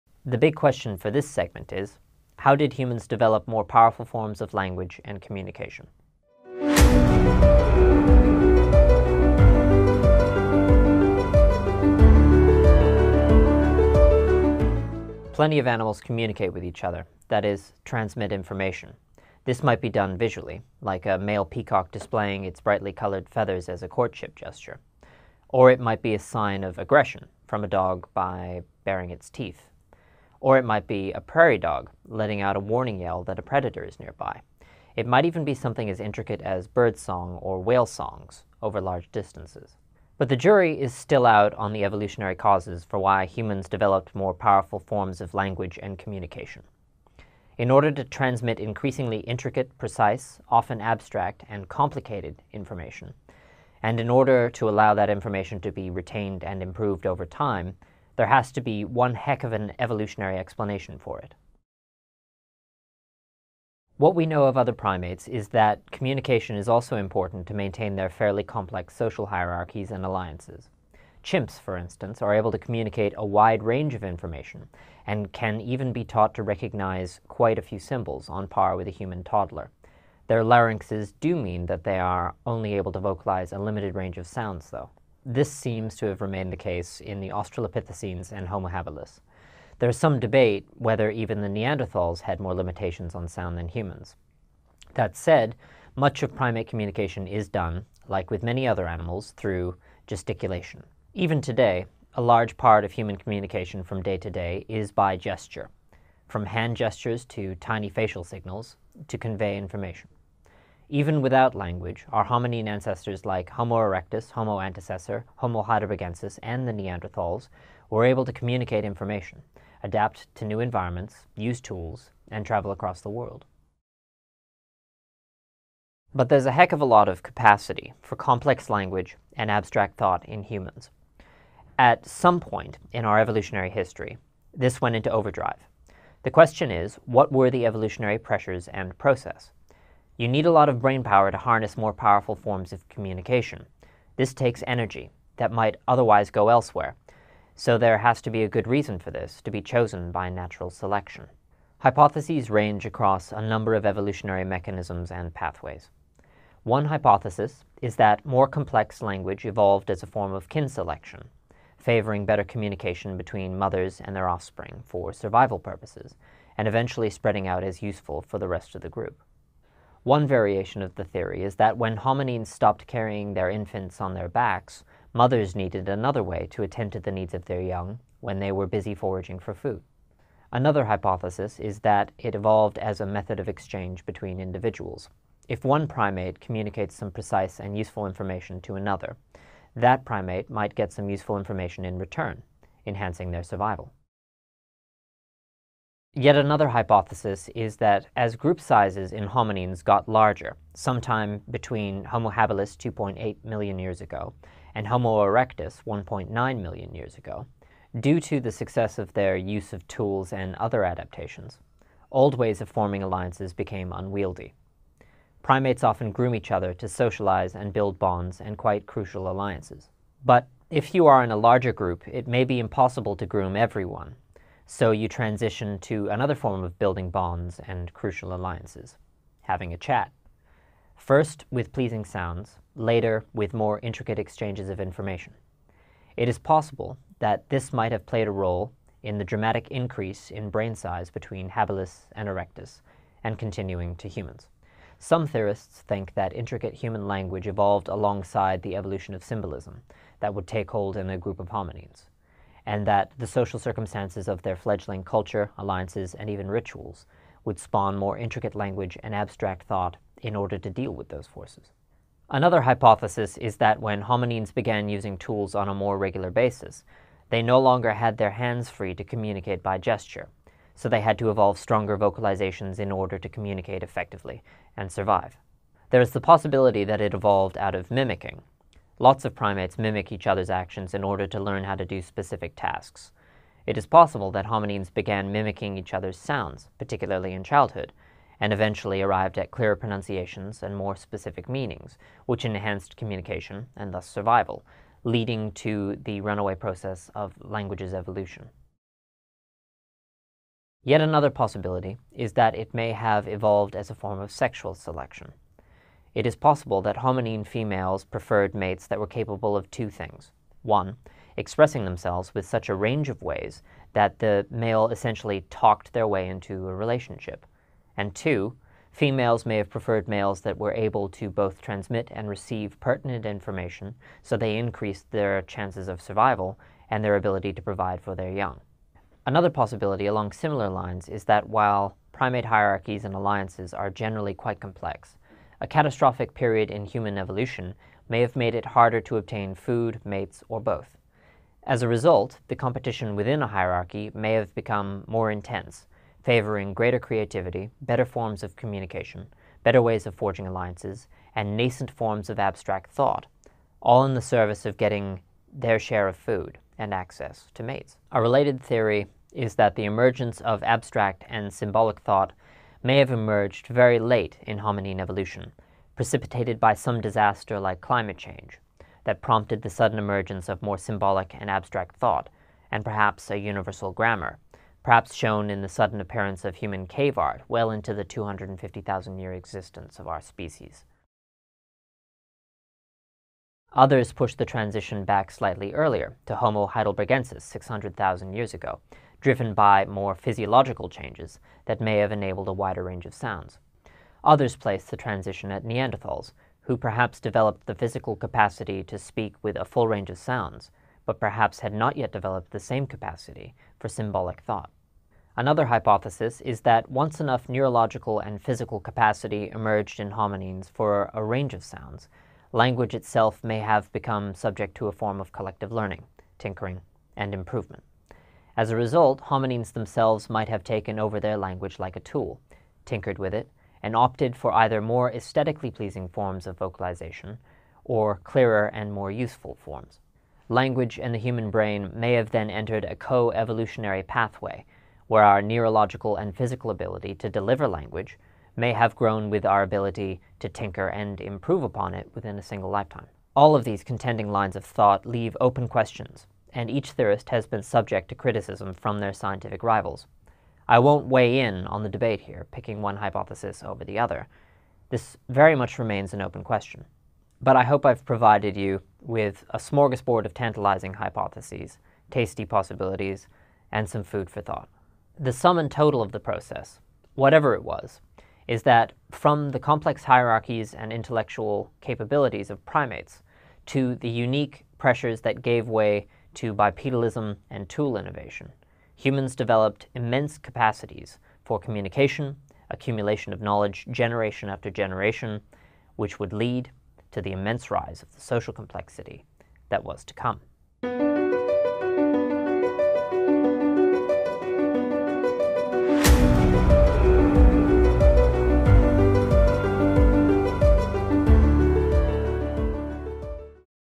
How did humans develop more powerful forms of language and communication? The Evolution of Language
Read

Robin Dunbar, Grooming, Gossip, and the Evolution of Language, (Cambridge, Mass: Harvard University Press, 1998).

Watch

Crash Course Big History: Human Evolution

Facts about Human Evolution

The Rise of Humans

Glossary

Australopithecines: Various species who were among the first bipedal apes in East Africa 3-4 million years ago, of which the most famous specimen is "Lucy" from the species Australopithecus afarensis.

Collective Learning: The ability of a species to accumulate more learning with each passing generation than is lost by the next.

Gesticulation: The communication of information through gesture rather than vocalisation. Present in many species and still playing a huge role in human communication today.

Homo antecessor: An extinct human ancestor that appeared 1.2 million years ago that displays an impressive capacity for adaptation, possibly a capacity for innovation, and possibly also spoke a restricted form of language.

Homo erectus:An extinct human ancestor that appeared 1.9 million years ago with an enhanced brain size and one of the first to successfully to spread across much of the old world adapting to various environments.

Homo habilis: An extinct human ancestor that appeared 2.1 million years ago that became notable for an increased use of tools compared to earlier species (that nevertheless made fairly frequent use of tools, an capability also demonstrated by our evolutionary cousins today).

Homo neanderthalensis: A recent evolutionary cousin that only went extinct 40,000 years ago that interbred with various groups of Homo sapiens while they both existed. The neanderthal shared approx 99.7% of our DNA and possessed many familiar capabilities for adaptation and perhaps even innovation.

Kin selection: Where a trait that has potentially negative or indifferent effects on an individual nevertheless improves the survival chances of a close gene pool, thus still fulfilling the goal of DNA replication for the close family, whether it be relatives or offspring.

Natural selection: Where advantageous traits that promote survival and reproduction are carried on to subsequent generations and potentially spread, while disadvantageous traits are non-randomly eliminated.

Sexual Selection: Where traits that may not necessarily promote survival for an individual are preferred by a potential mate and are thus naturally selected for due to successful reproduction.